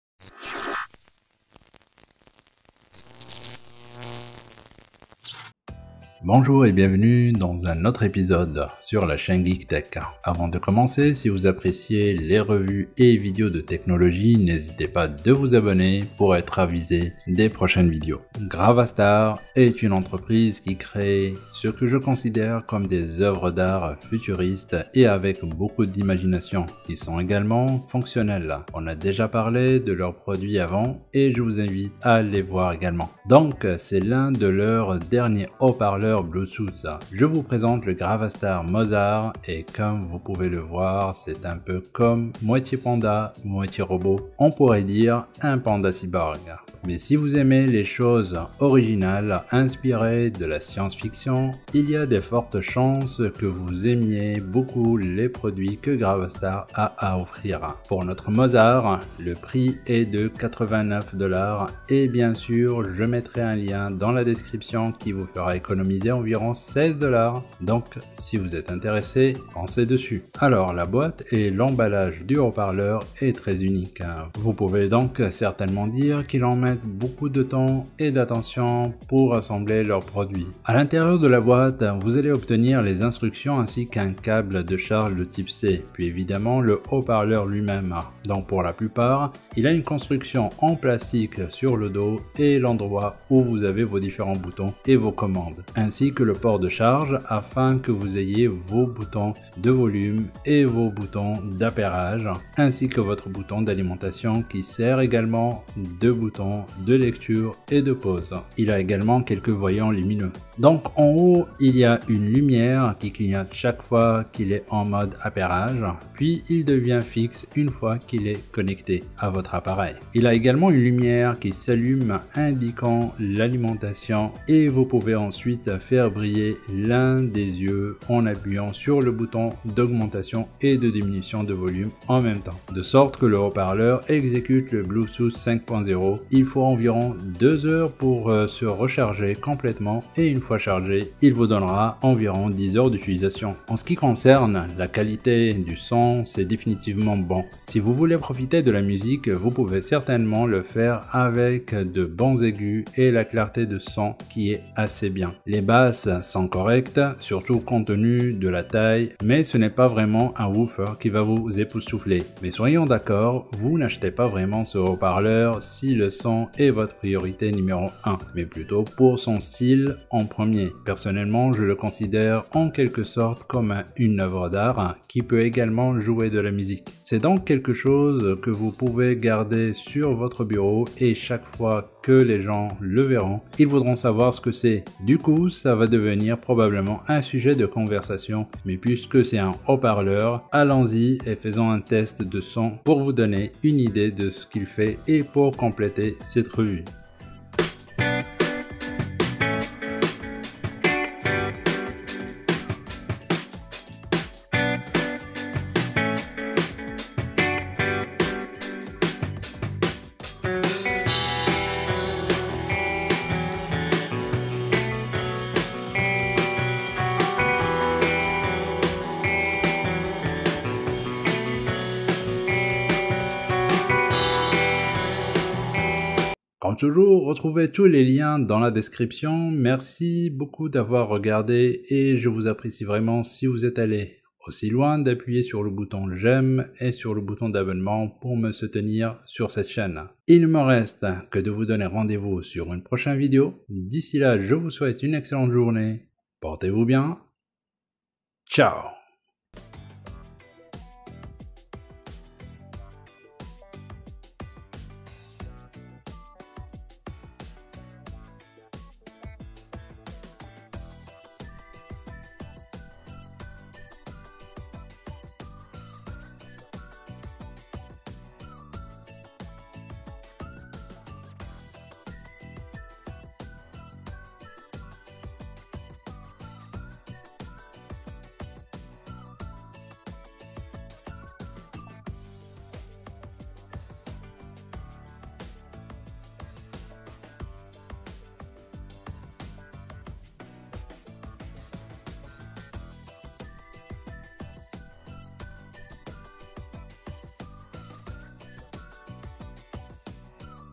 Un nouveau haut parleur du futur → Gravastar Mozart
Cyberpunk | @Gravastarofficial a déployer le Gravastar Mozart 2022 Haut-parleur venu de l'espace.
Pas seulement un panda mecha de science-fiction futuriste, mais aussi une enceinte Bluetooth. Un son équilibré raconté à travers un design cyberpunk.
Gravastar présente sa nouvelle enceinte futuriste Mozart, un panda robot qui délivre un son incroyable et si vous en avez deux c'est encore mieux, on peut profiter du son surround. Si vous voulez en savoir plus, je vous laisse le lien :

Voici le lien d'achat et le code de réduction de 16% :
Code de réduction de 16% : GEEKTECH